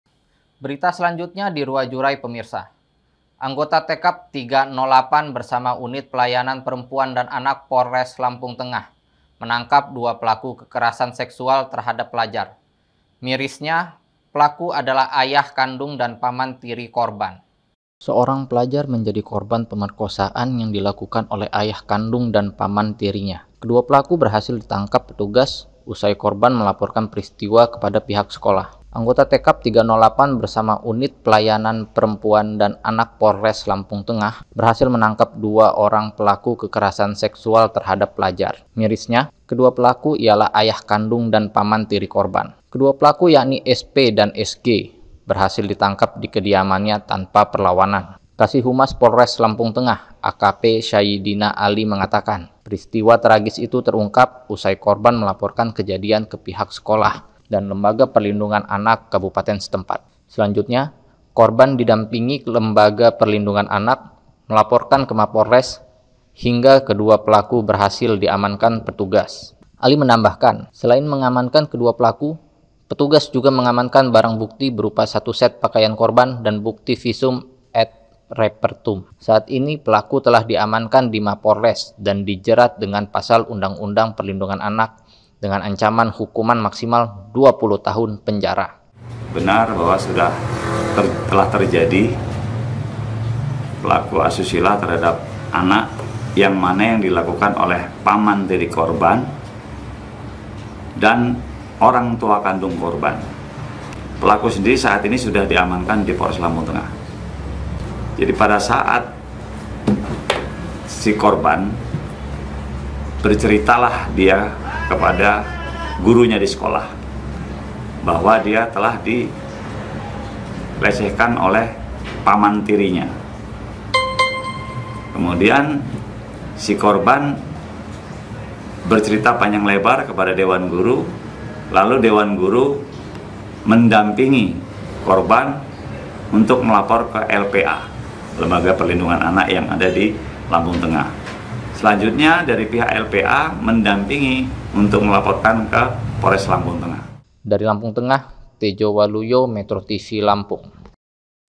POLISI TANGKAP AYAH DAN PAMAN PELAKU KEKERASAN SEKSUAL DI LAMTENG
Anggota Tekab 308 Bersama Unit Pelayanan Perempuan Dan Anak Polres Lampung Tengah menangkap dua pelaku Kekerasan Seksual terhadap pelajar// mirisnya/ pelaku adalah ayah kandung dan paman tiri korban.